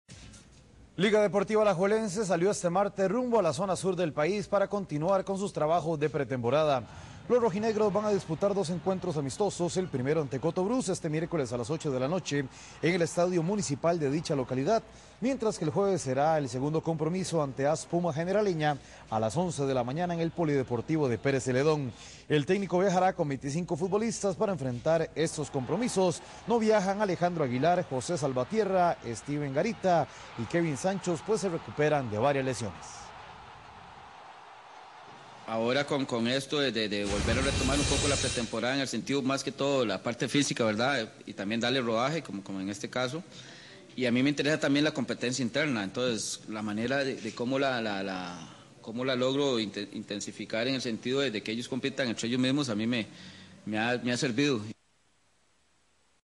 Alajuela viaja al sur para seguir su Pretemporada
lo liguistas viajaron al sur para seguir su preparacion para el inicio del torneo